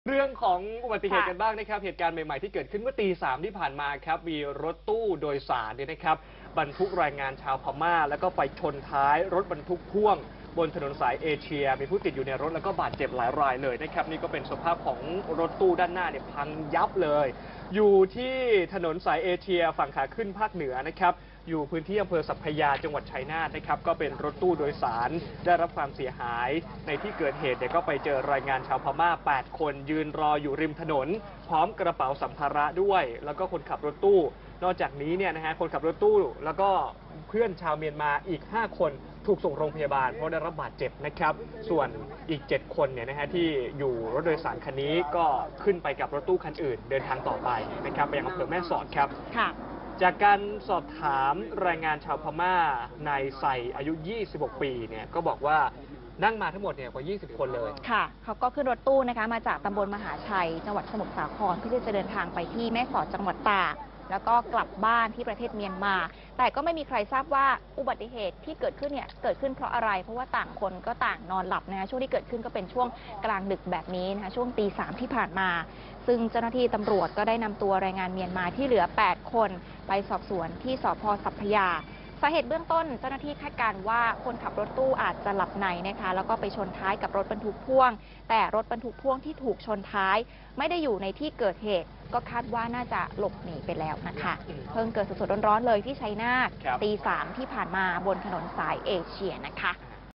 รถตู้โดยสารขนแรงงานเมียนมาร์ชนท้ายรถพ่วง เจ็บ 6 | สำนักข่าวไทย อสมท
ชัยนาท 3 เม.ย.-รถตู้โดยสารขนแรงงานเมียนมาร์พุ่งชนท้ายรถบรรทุกพ่วงบนถนนสายเอเชีย บาดเจ็บ 6 คน สาเหตุเบื้องต้นคาดคนขับรถตู้หลับใน.-สำนักข่าวไทย